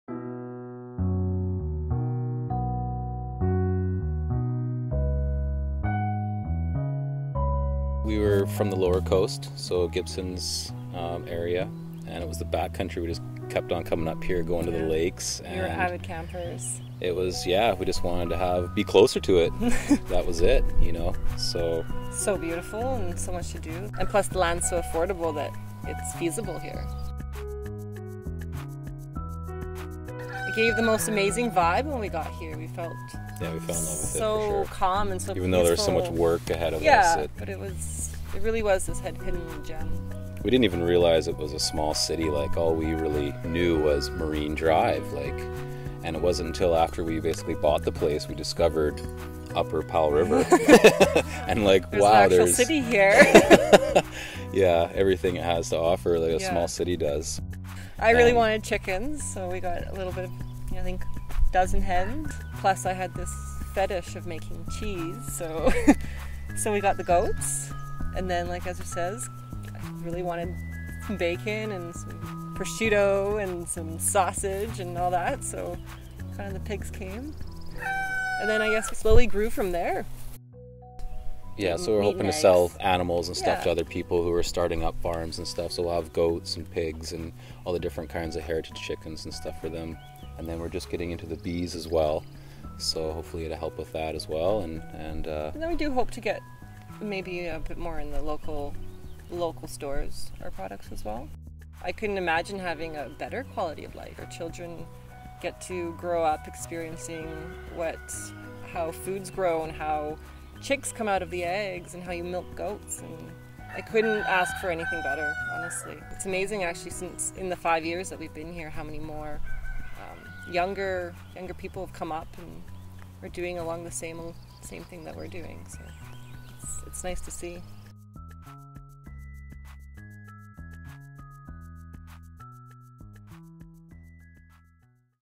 Ezra and Gosia
Self-sufficiency in addition to gardening are growing interests for young couples. Ezra and Gosia, who own Myrtle Point Heritage Farm, didn't just find affordable land to establish a small-scale farm, they also found a supportive community too.